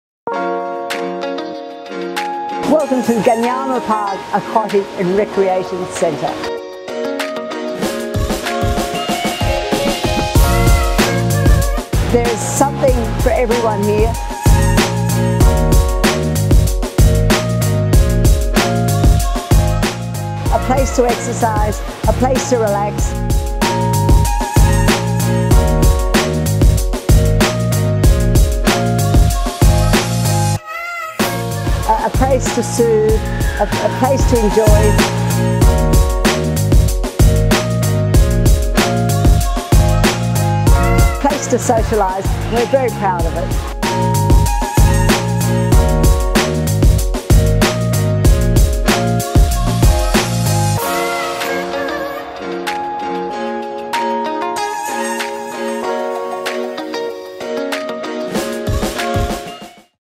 Gunyama Park Aquatic and Recreation Centre is now open
Shows all the facilies at Gunyama Park with a welcome from Sydney's Lord Mayor, Clover Moore.